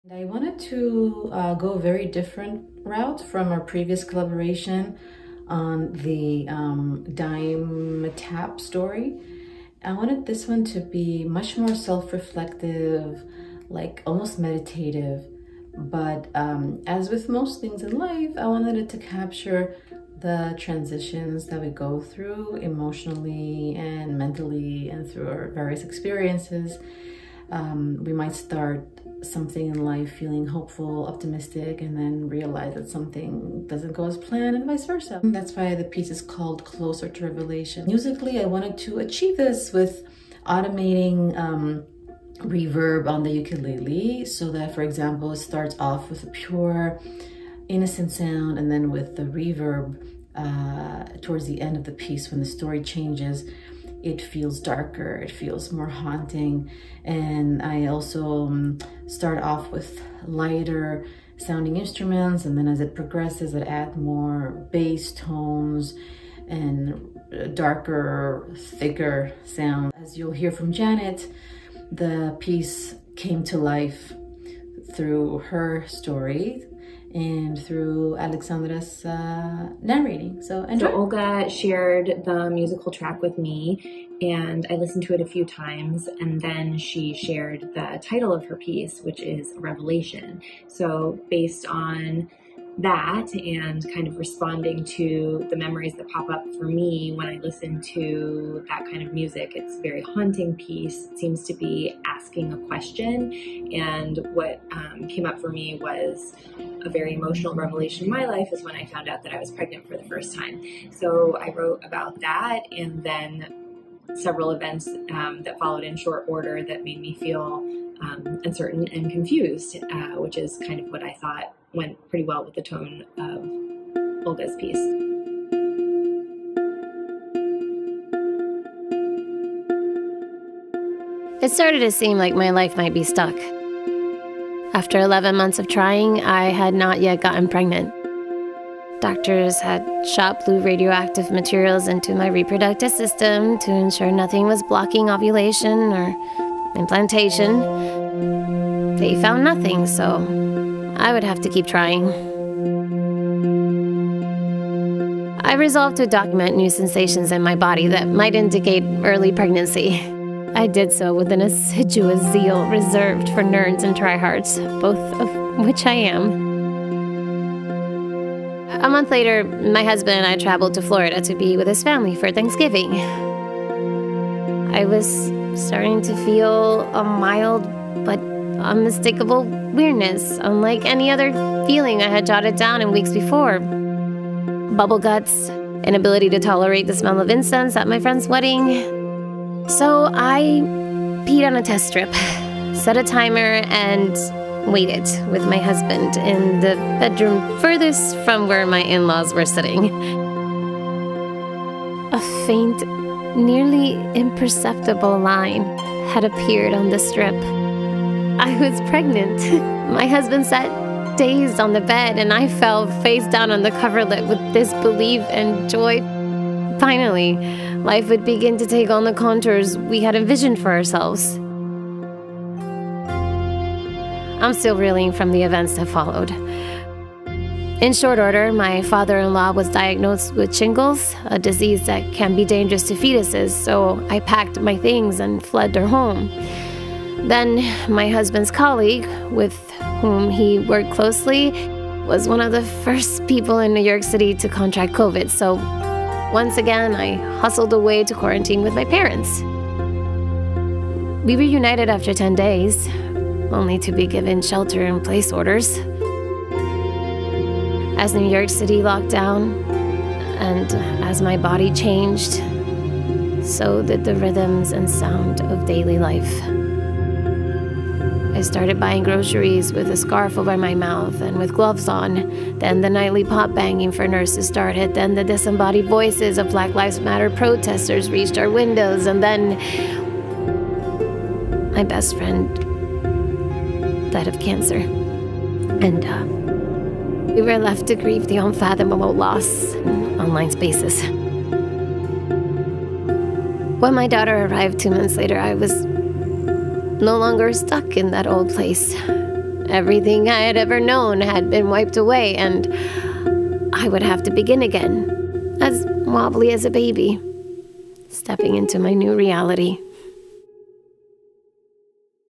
"Close to Revelation": Personal Narrative Story
Non-fiction, personal narrative story about difficult and major transitions in young adulthood.
Narrated by @PuertoRico2170 Alexandra Echavarri
Original score by Olga Gonithellis